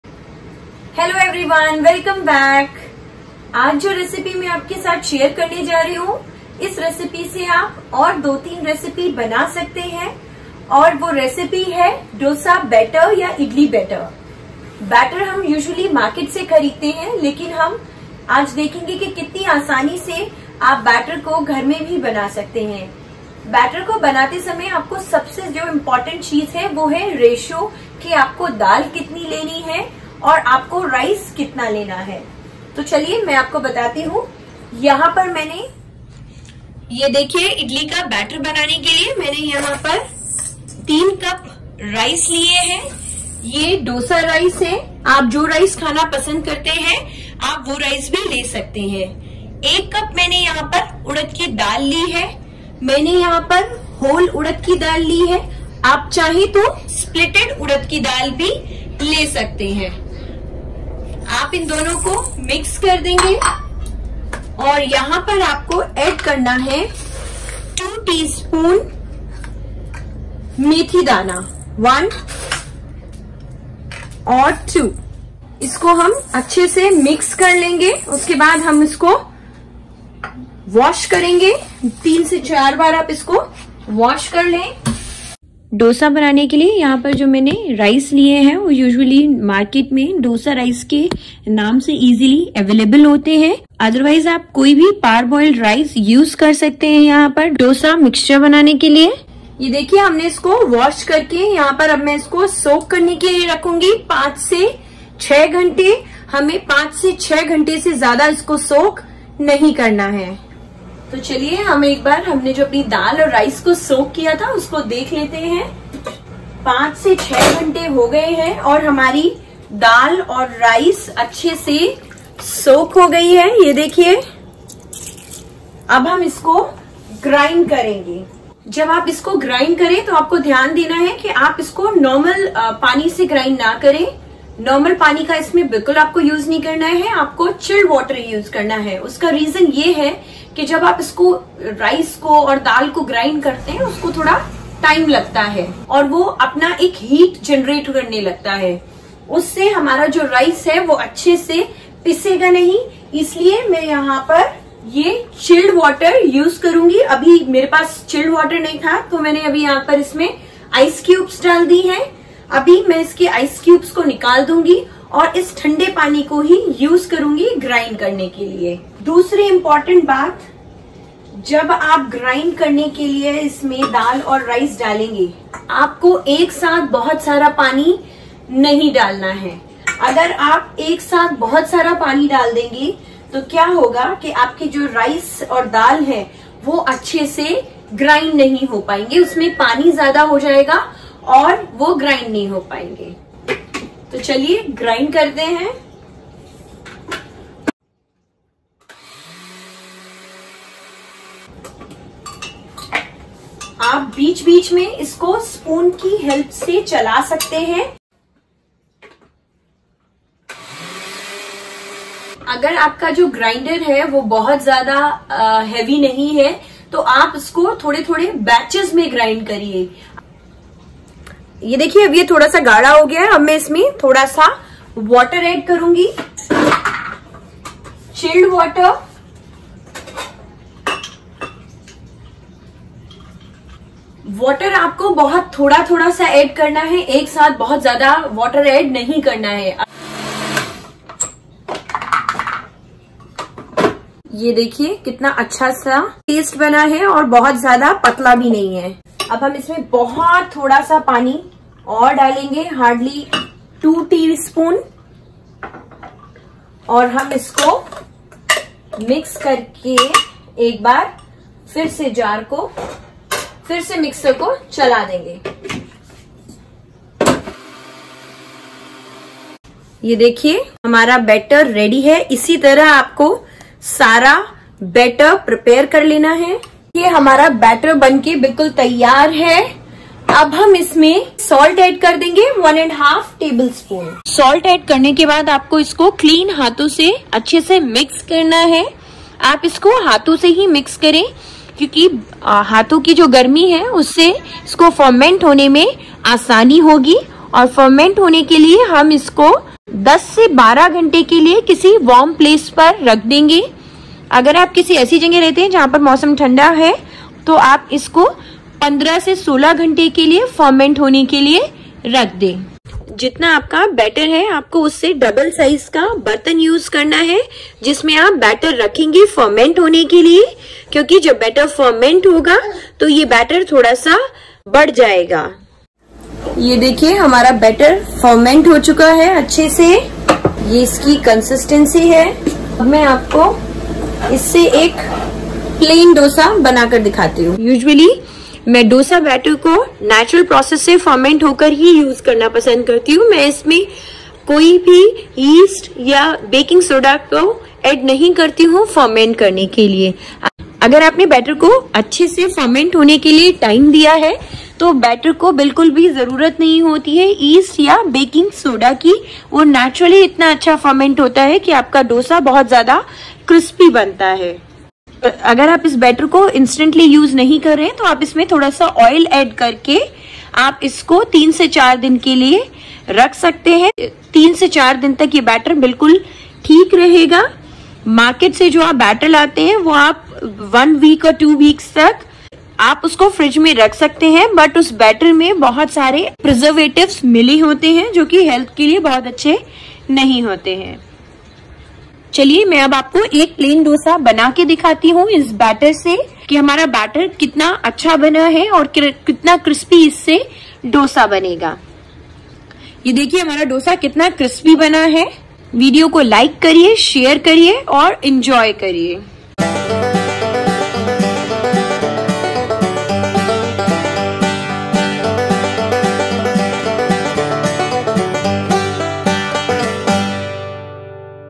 4 in 1 Breakfast Batter | Dosa Batter in Mixie | Homemade Crispy Dosa Batter Recipe
Dosa Batter
Ingredients
Parboiled /Dosa Rice 3 cup
Urad Dal 1 cup
Methi dana 2 tsp
Salt 1.5-2 tbsp

Instructions
In a bowl add rice, dal and methi dana. Wash it then thoroughly 3-4 time and soak it for for 5-6 hours.
After soaking put soaked ingredients into mixer and grind them finely in batches by adding cold water as needed.
Put all the batter in a big pot and add salt, mix this batter by clean hand. This will help fermentation process. Always use big pot for fermentation to avoid spill because the batter will rise after fermentation.
Let this batter set aside to ferment for 10-12 hours in a warm place.
If you live in a warm place keep it for 15-16 hours for fermentation.
This batter is good for 3-4 days. Add little oil in batter if you are keeping it for later use.